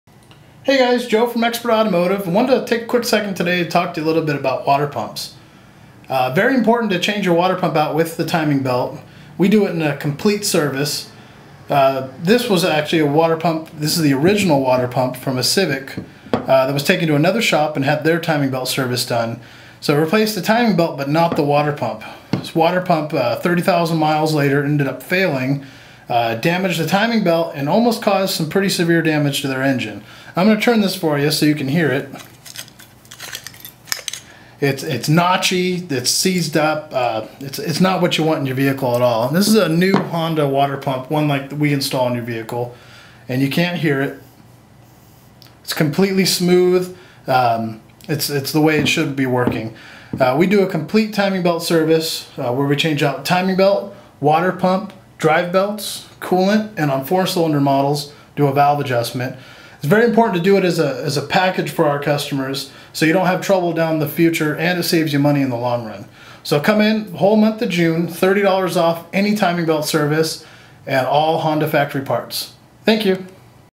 Water Pump and Timing Belt
Why we replace the water pump when we replace the timing belt and why it matters.